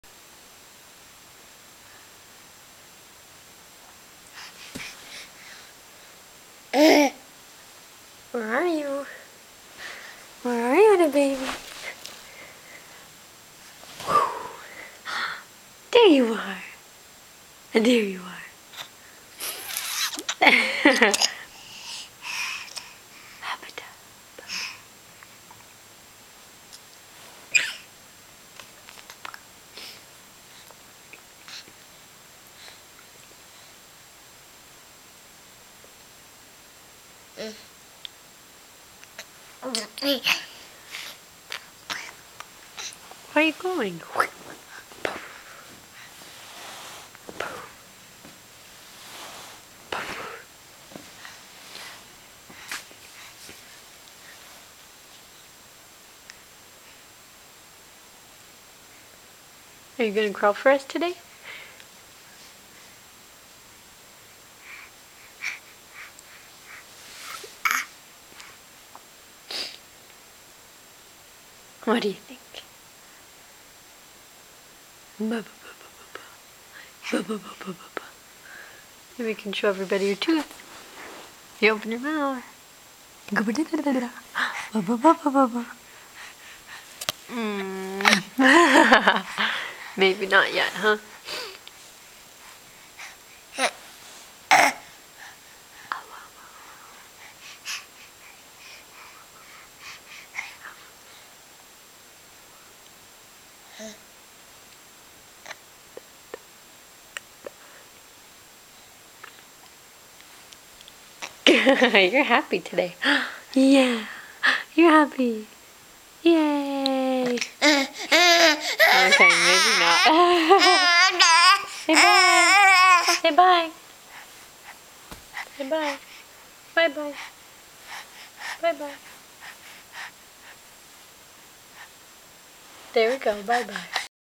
Scoochy-Coo
This is before Aedan could crawl. He was scooching around.